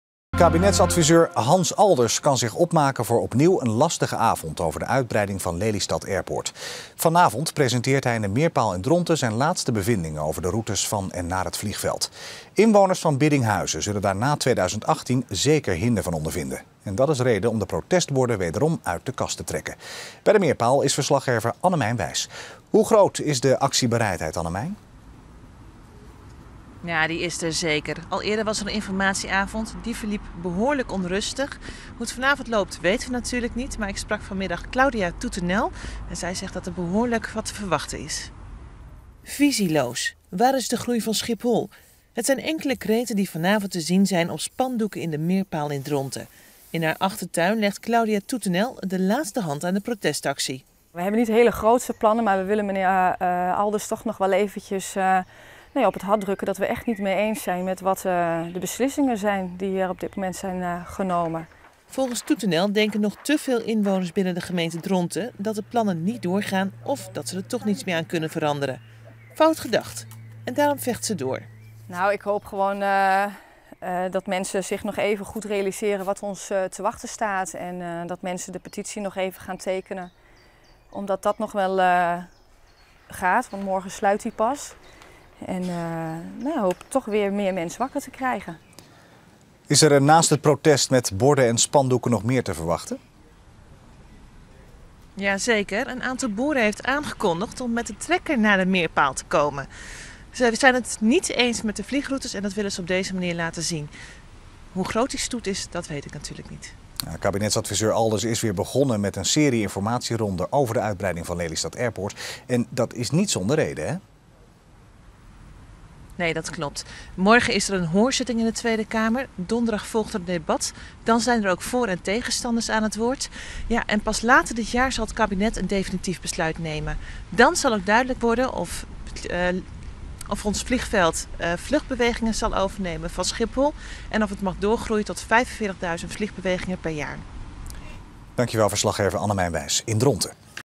24 juni 2014: Tractoren in optocht naar Alders
Een stoet van tractoren uit Biddinghuizen gaat dinsdagavond in optocht naar de Meerpaal in Dronten. Inwoners van het dorp wonen de informatie-avond bij van kabinetsadviseur Hans Alders over de uitbreiding van Lelystad Airport.